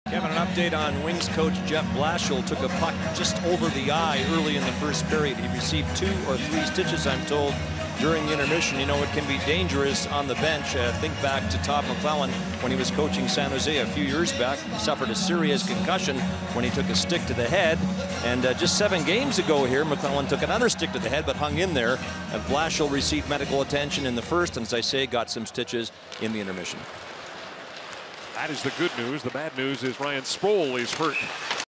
Blashill receives stiches after puck goes flying into bench
Jeff Blashill needed some repairs after a puck flew into the Red Wings bench.